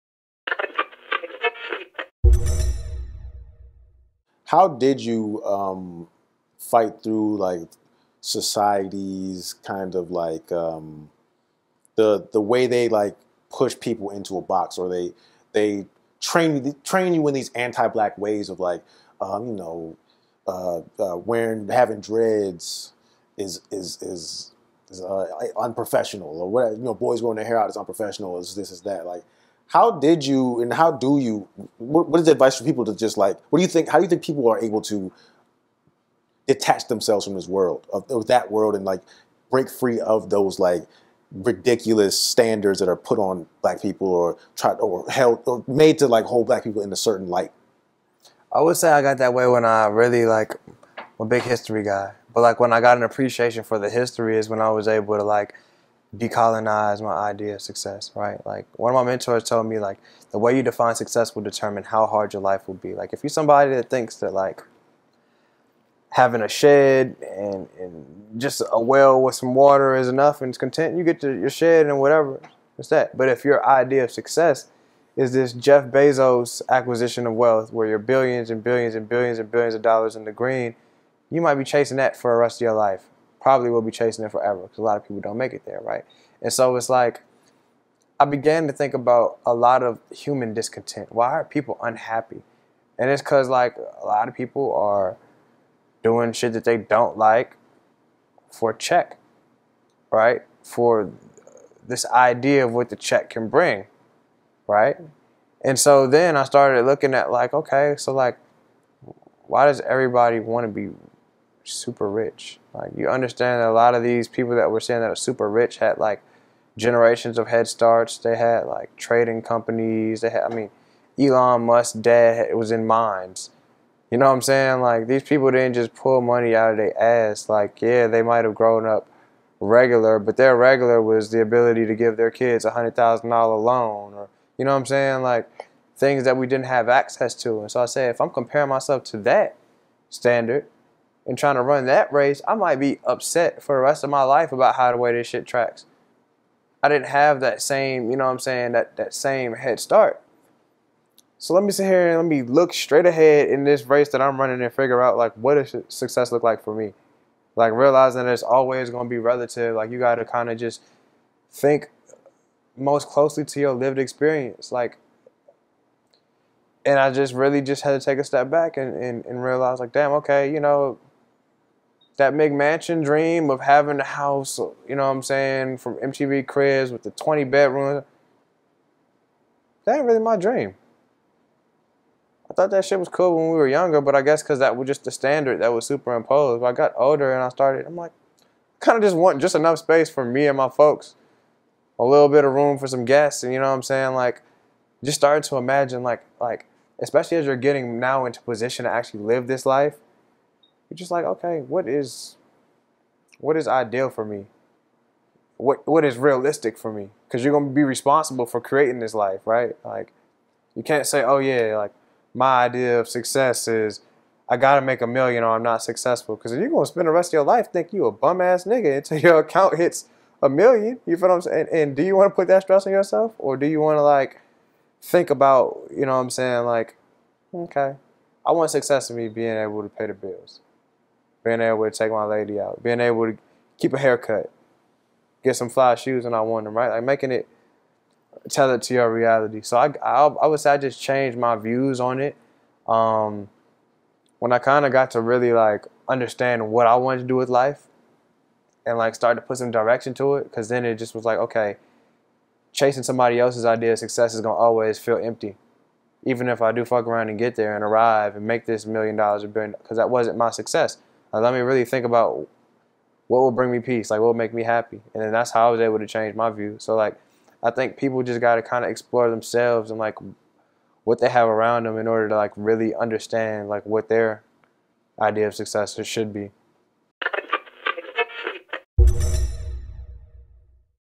Ty shares his views on what "success" should mean, focussing on realistic goals (Part 4)
Ty talks about avoiding societal standards of success, creating his own definition of success.